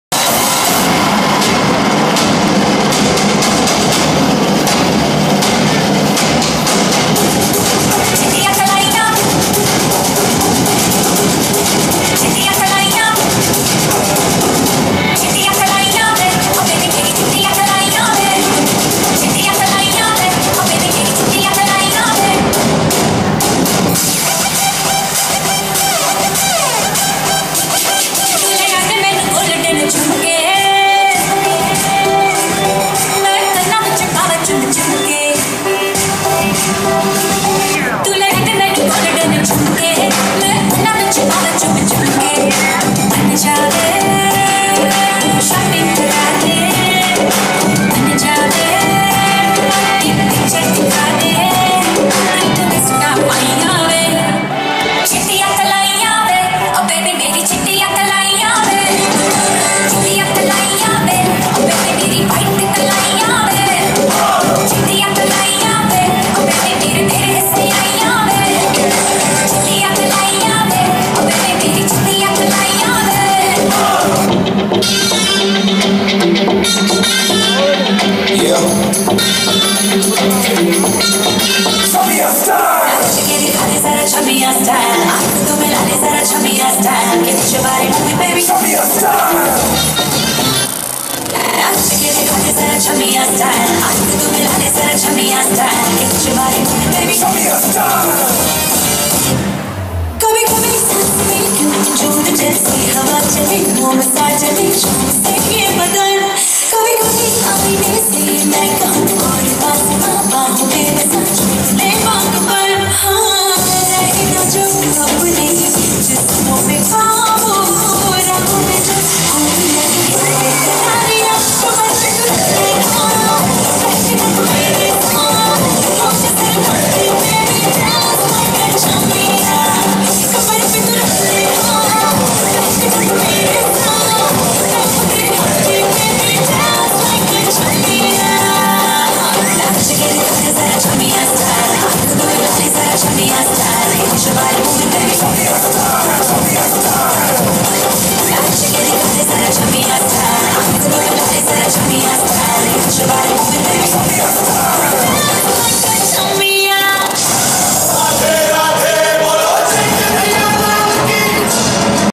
Diwali Mela - Edison NJ 2016 - Kavi school of dance
Ayana Tyagi & Sahana Tyagi - Diwali Mela - Edison NJ 2016 - Kavi school of dance